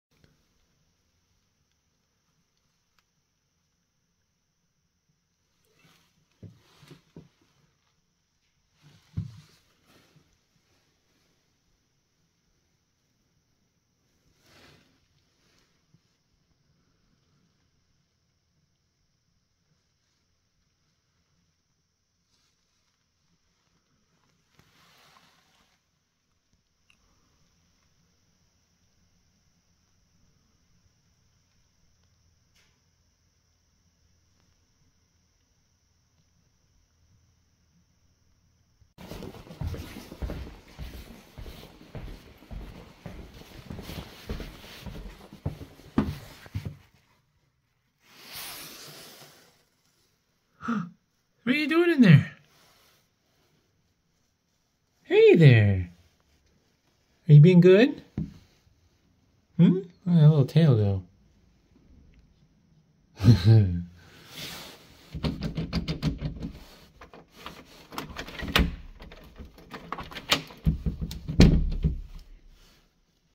Additional footage of Smokey from the room swap on 1-27-24
Didn't add it to the video, but uploading it for record's sake.